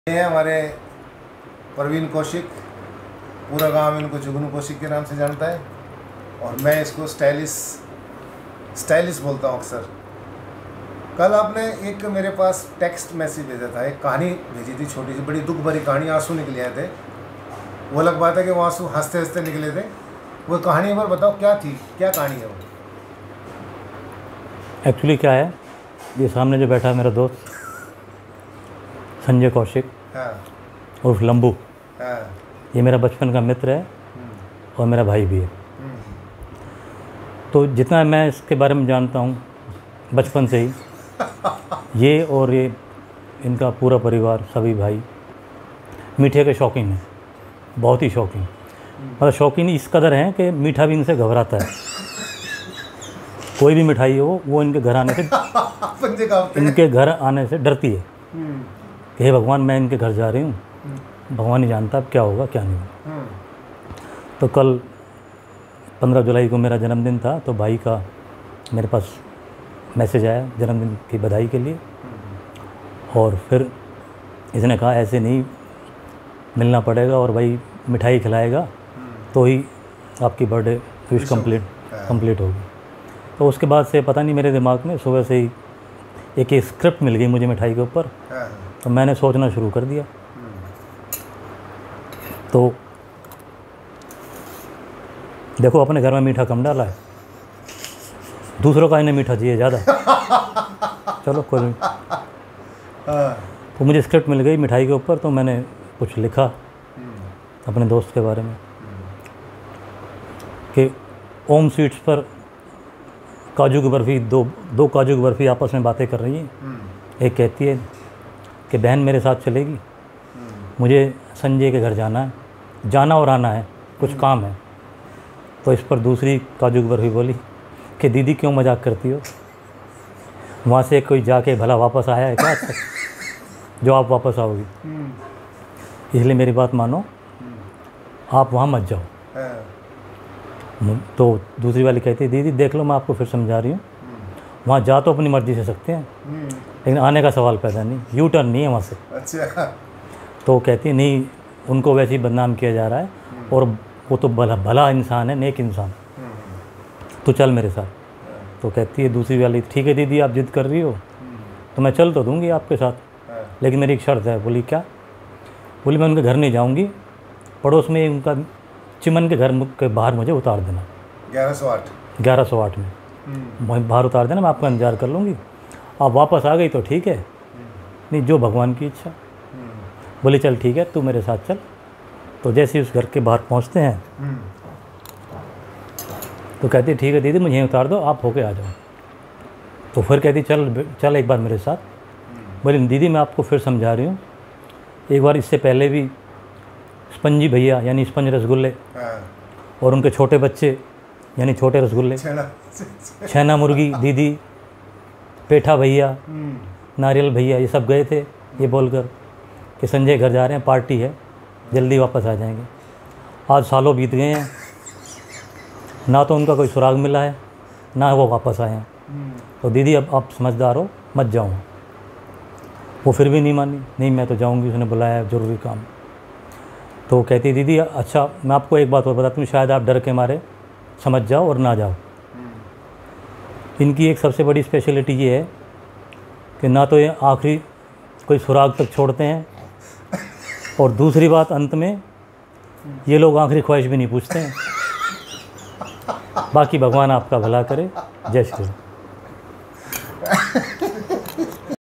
आख़िरी ख्वाईश | हांस्य से भरपूर एक दुख भरी दास्ताँ
काजू कतली की दुख भरी दास्ताँ | funny story by Praveen Kaushik

दर्द भरी दास्तां-
एक बार 2 काजू की बर्फ़ी आपस में बात कर रही थी , बहन मेरे साथ चलेगी क्या मुझे संजय के घर जाना और आना है थोड़ी देर का काम ह, इस पर दूसरी काजू बर्फ़ी बोली दीदी क्यों मज़ाक़ करती हो वहाँ से कोई वापस आया ह क्या भला , दीदी वहाँ जा तो सकते ह पर वापस आने का तो कोई सवाल ही भी दीदी इसलिए मेरी बात मानो और आप भी मत जाओ , पहली वाली कहती ह के ऐसी कोई बात नहीं ह वो बहुत नेक इंसान ह तू चल मेरे साथ , ठीक ह दीदी आप इतना ज़िद कर रही हो तो मैं चल तो दूँगी पर मुझे चिम्मन के घर के बाहर उतार दैना मैं वहीं तुम्हारा इंतज़ार कर लूँगी पर मैं वहाँ नहीं जाऊँगी , मैं फिर आपसे कहती हूँ की इस से पहले भी स्पंज़ी भई और उनके छोटे बच्चे , नारियल भाई साहब, पेठा भैया छैना और उनकी बहने ये सब भी बोल कर गये थे की उनके घर जा रहे है कोई पार्टी है जल्दी वापस आ जायेंगे, लेकिन कई साल बीत गये आज तक वापस नहीं आये, बाक़ी काजू दीदी आपकी मर्ज़ी ह ,आप जाना चाहो तो…
अंत में सबसे भयानक बात और बताने जा रही हूँ
ये लोग तो आँखरी ख्वाहिश भी नहीं पूछते🤣🤣
आपकी शुभ चिंतक बेबस काजू बर्फी💐🙏🏻